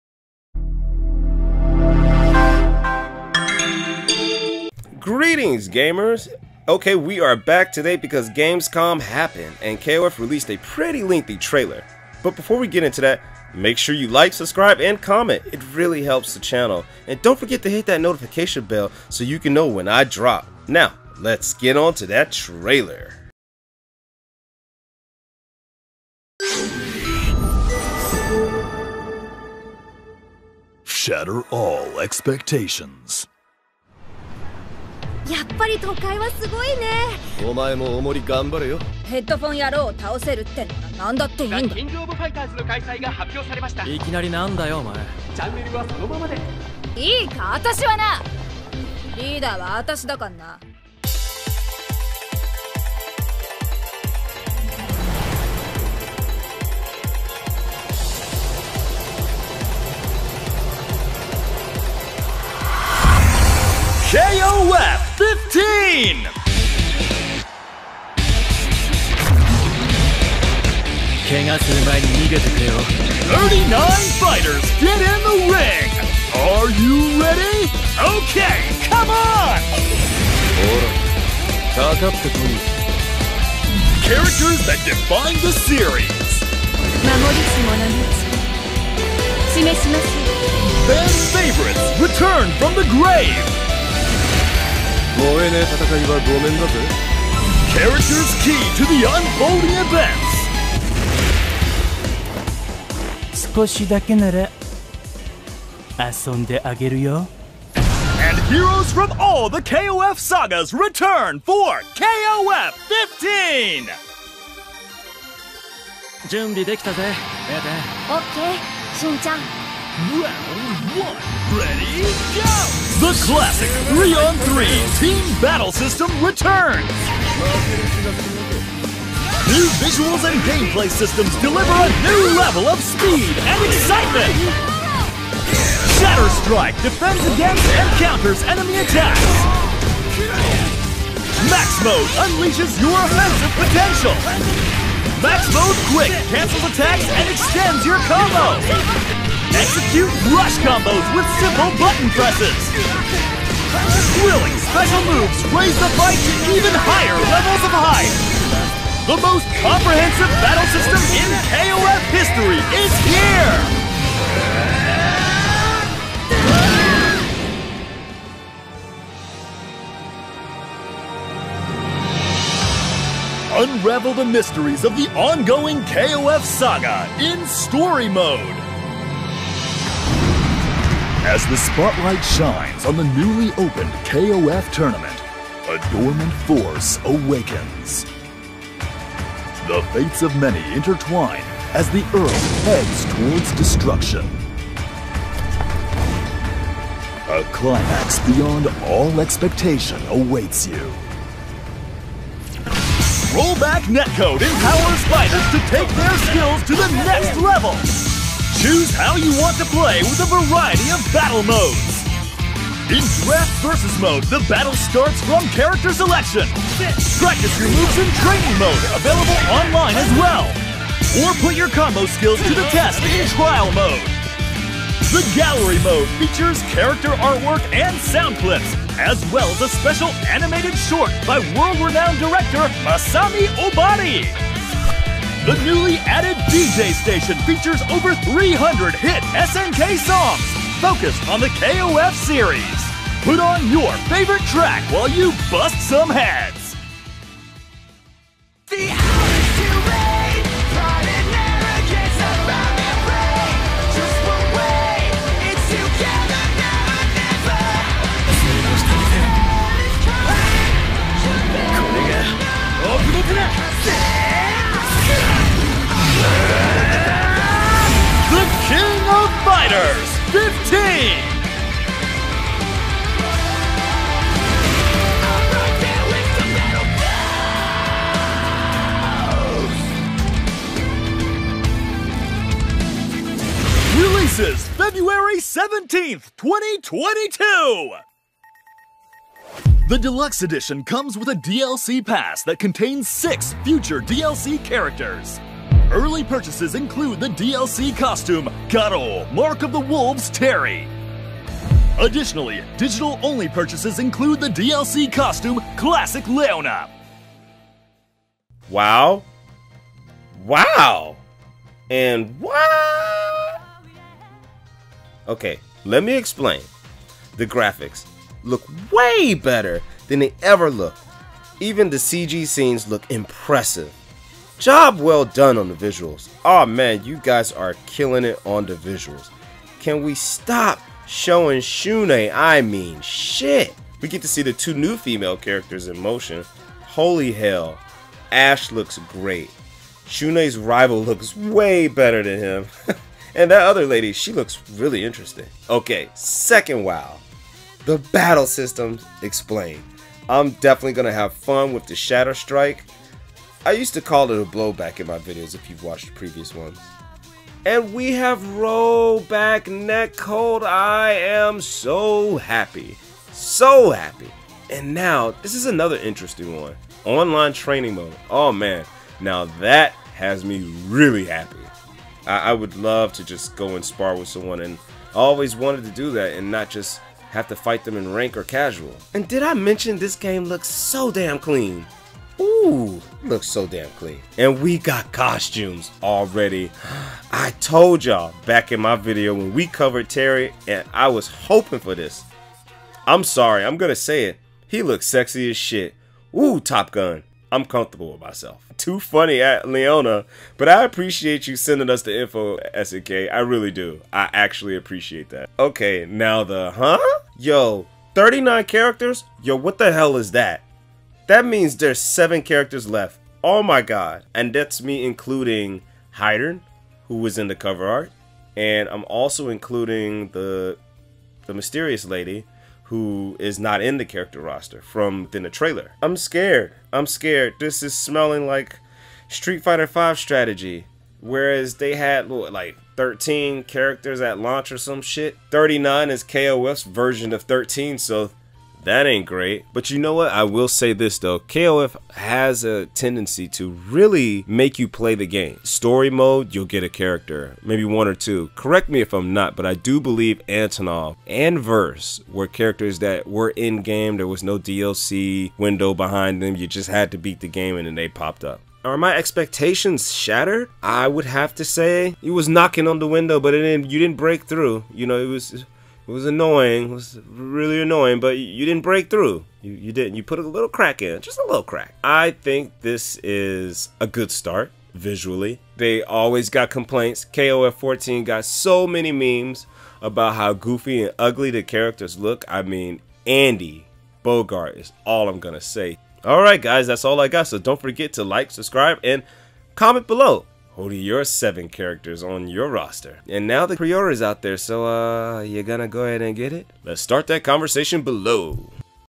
KOF XV GAMESCOM DEFEINITIVE TRAILER REVIEW
Greetings Gamers and we are back!

I am excited and confused. Looks like SNK shut it down at the Gamescom event showcasing new in game footage and revealing new and old characters.

We finally get to see the HUD!!!

I am a bit nervous because Angel has yet to grace our presence.

Anywho it’s great to finally get some clarification on mechanics and DLC COSTUMES!?
It’s way too much to type about lol…

Find out what else is going on in the world of Public